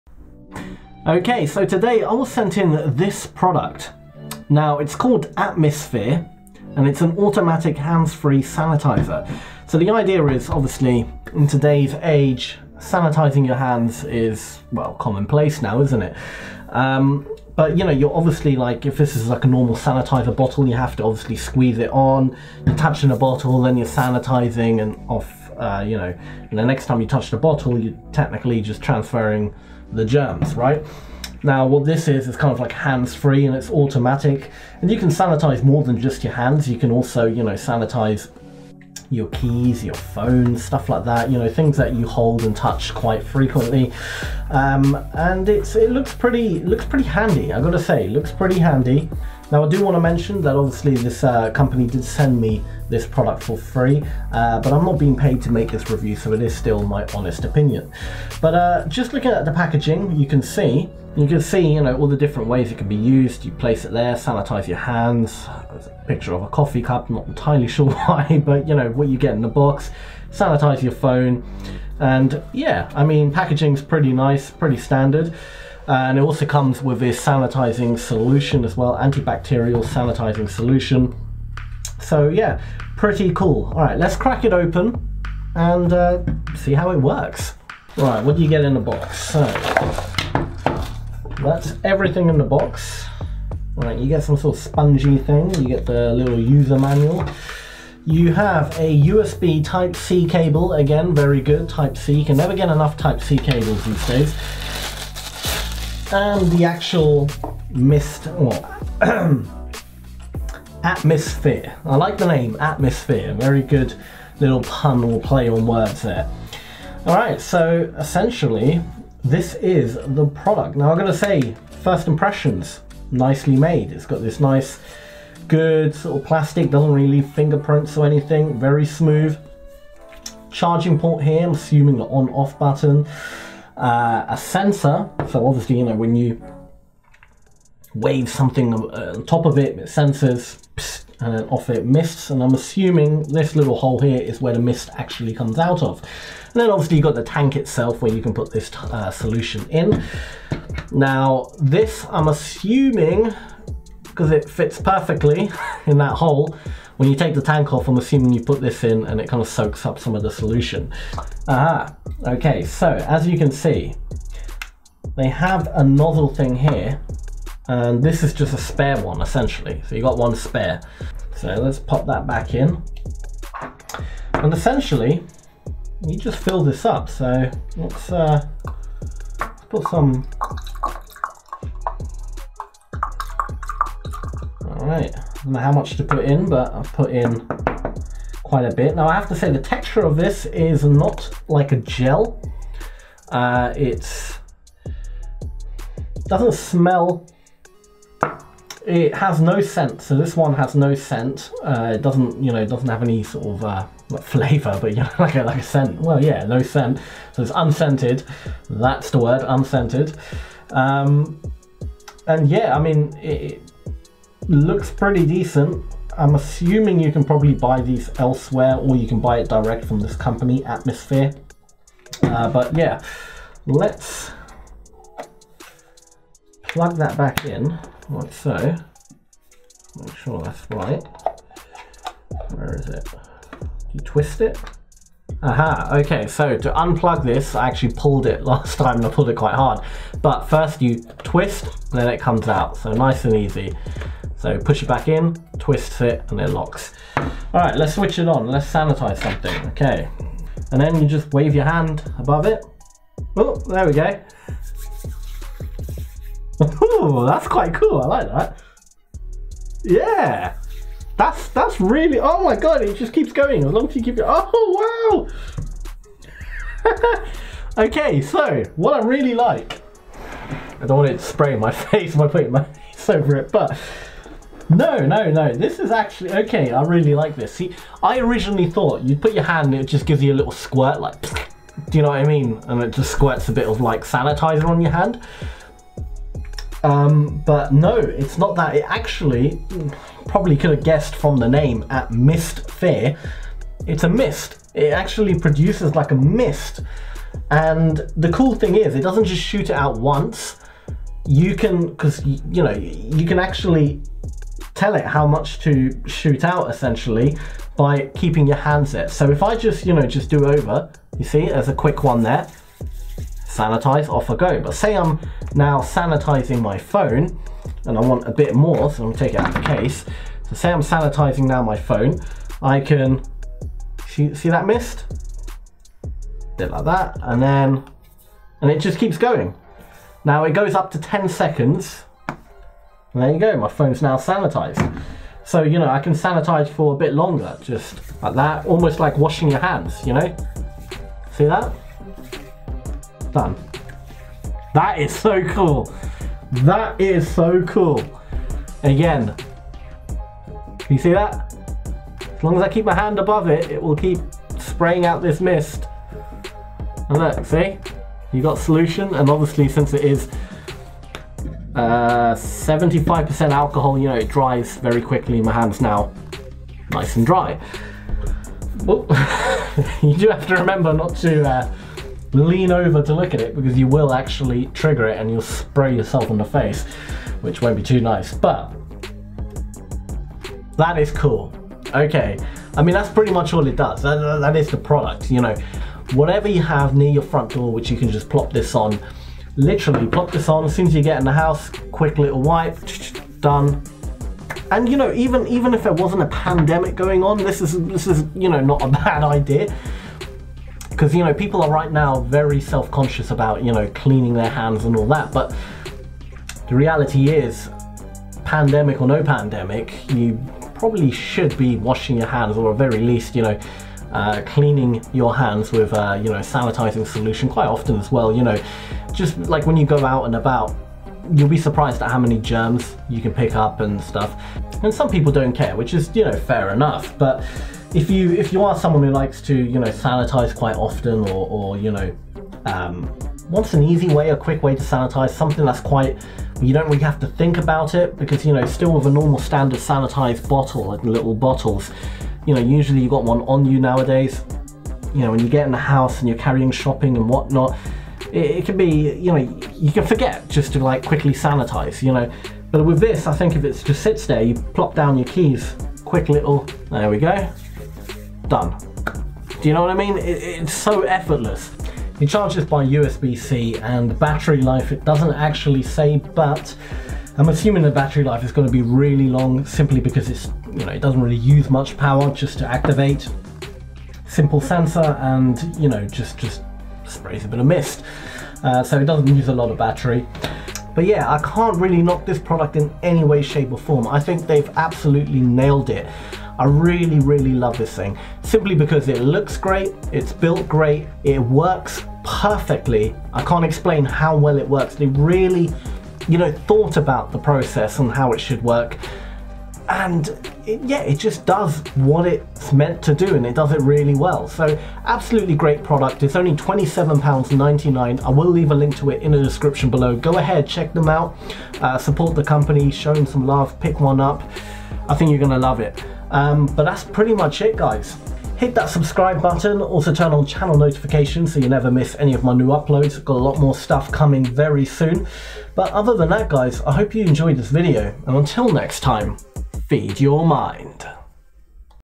MUST Have Tech For MODERN TIMES - Atmistphere Automated Hand Sanitizer
Today I test out the Atmistphere, a sleek contactless automated hand sanitizer that uses a sensor to detect your hands. Once detected it sprays a mist of hand santizer.

A simple yet really well made gadget that's a must in these modern times.